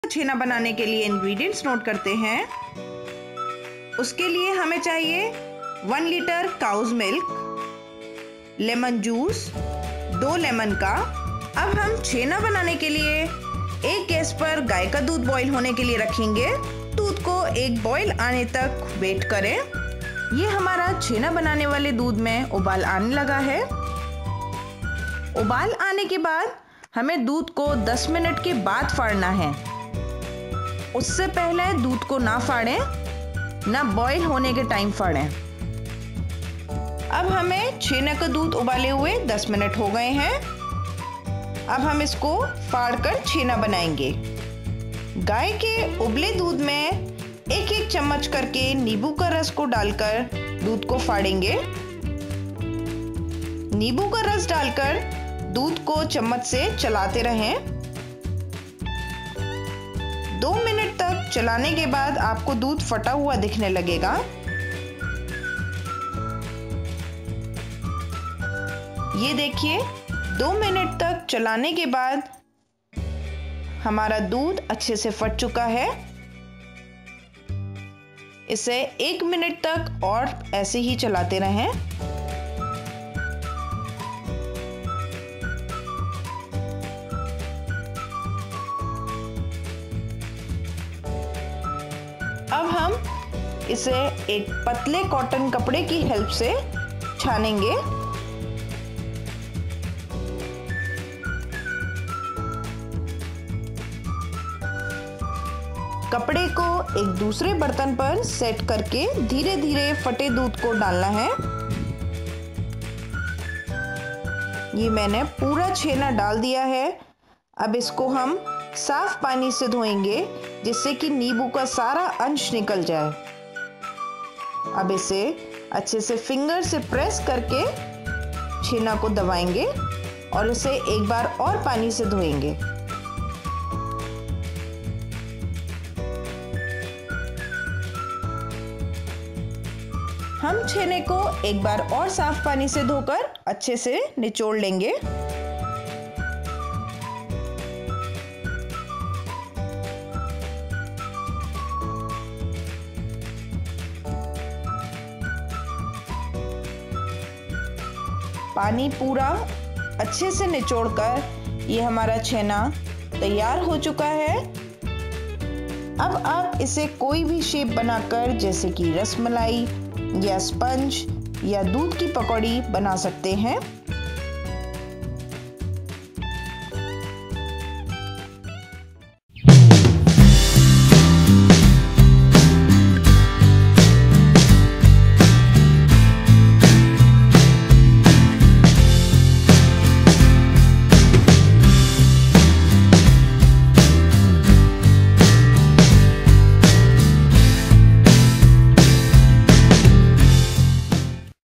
घर पे छैना बनाने की आसान विधि | छैना रसगुल्ला | How to make Chhena at home in Hindi
A short video on : घर पे छैना बनाने की आसान विधि  | छैना रसगुल्ला | How to make Chhena Rasgulla at home in Hindi . You can use छैना for preparing रसमलाई , सपंज , रसगुल्ला  and many similar Bengali Sweets.

You can get in touch with me by clicking on this

Thanks a tonne for watching it!

I hope that this video on घर पे छैना रसगुल्ला बनाने की आसान विधि  | How to make Chhena Rasgulla at home in Hindi  is liked by you.